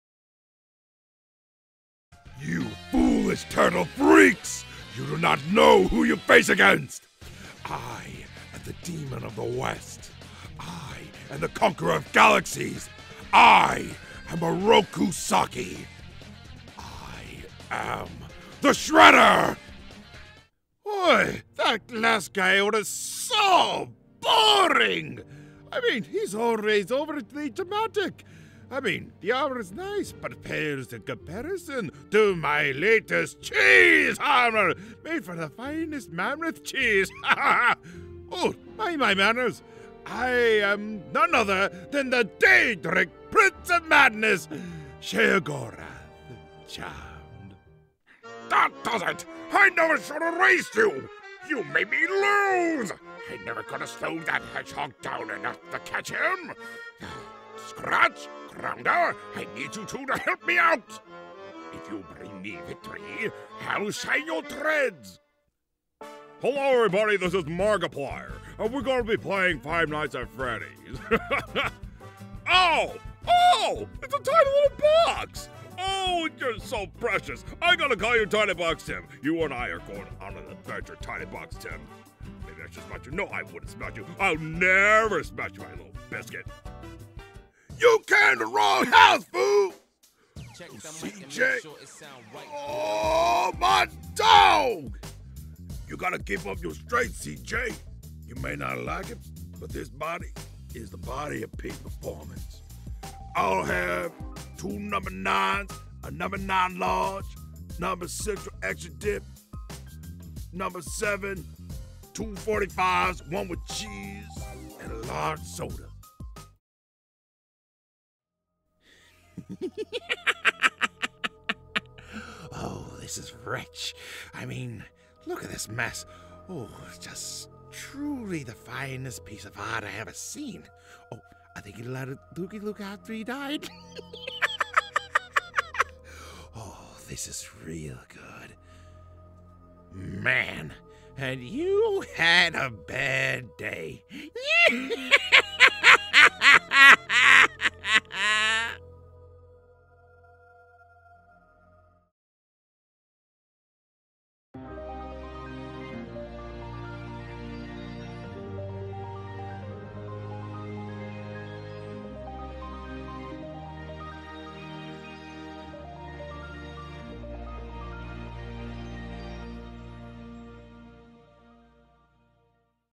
Characters to Voice (Bold King Cole)
Join the Stars

Bold King Cole is Voicing 6 Character Impressions of his choice.  i think you might like what you hear and more.

More Uploads to come in the next days